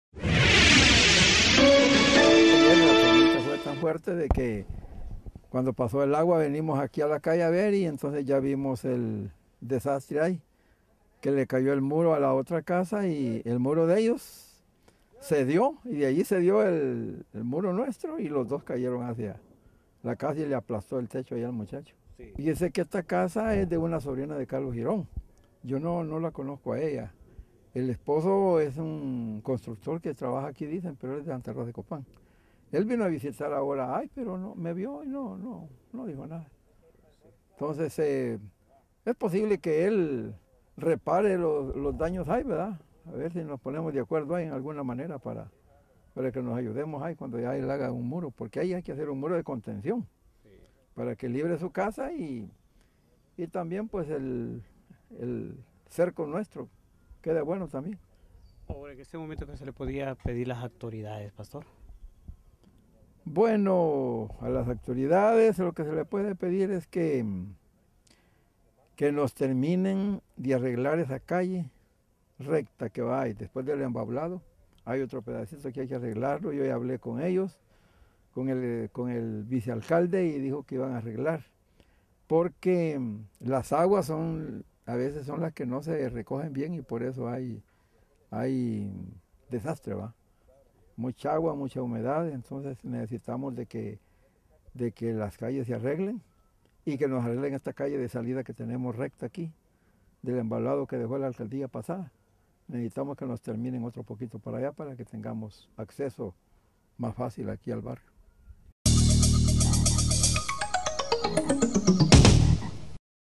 Avance Noticias san marcos tv_8 julio_ edición 1
Isaías flores, habitante del barrio Villa Florencia nos comenta acerca de un muro de su propiedad se derrumbo sobre otra casa...